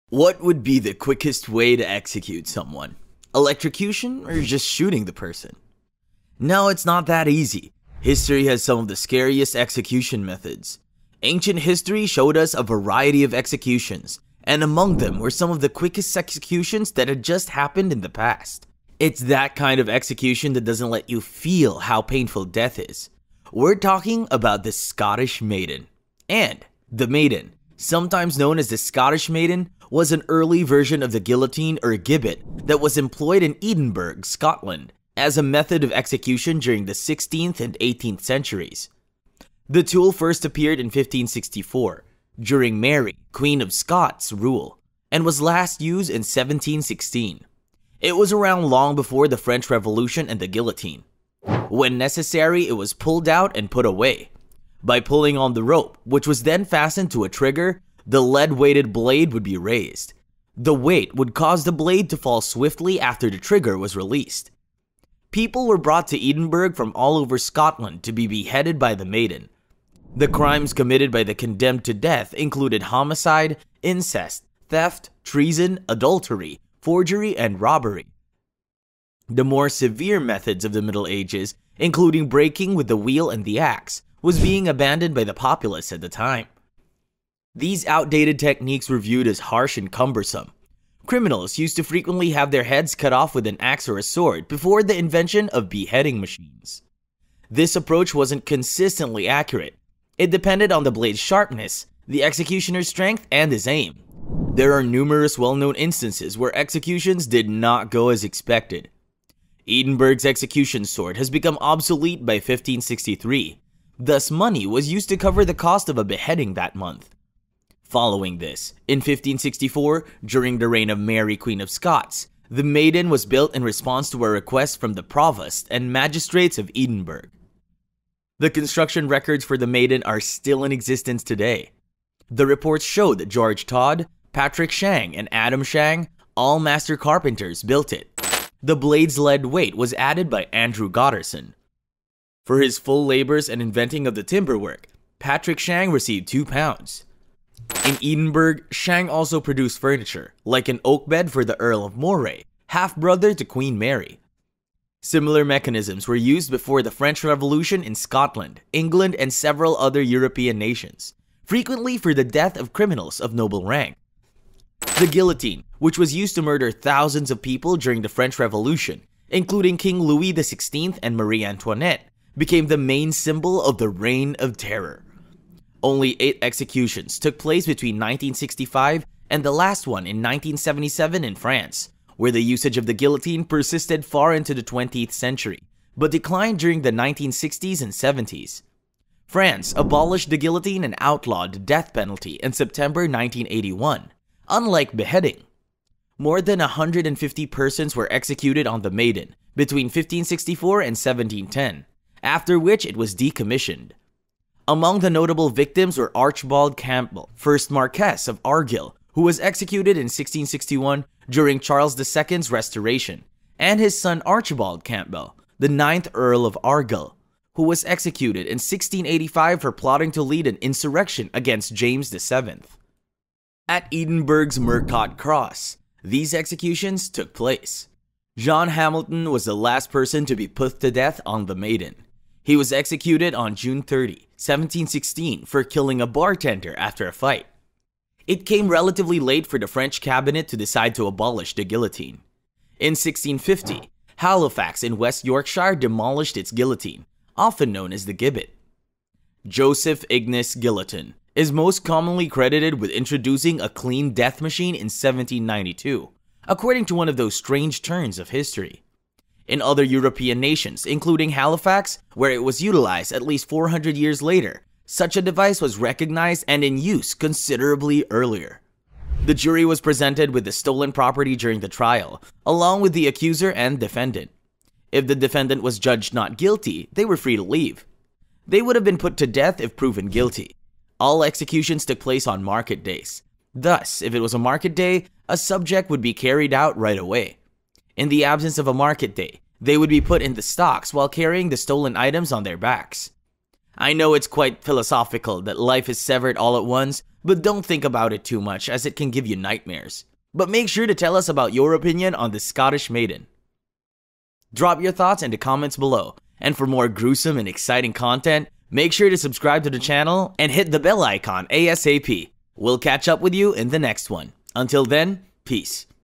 The QUICKEST Execution EVER
What would be the quickest way to execute someone? Electrocution or shooting the person?
No, it’s not that easy.. history has some of the scariest execution methods.. Ancient history showed us a variety of executions and among them there were some quickest executions that had happened in the past.
It's that kind of execution that doesn't let you feel how painful death is. We're talking about The Scottish Maiden.